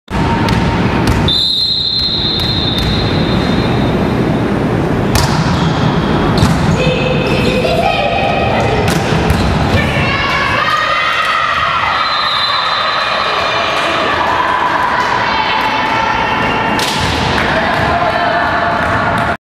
Fanelli (attacco).MP4
attacco vincente di Giulia Fanelli centrale della Ars Perugia di serie C volley femminile stagione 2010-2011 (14^ giornata di campionato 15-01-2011)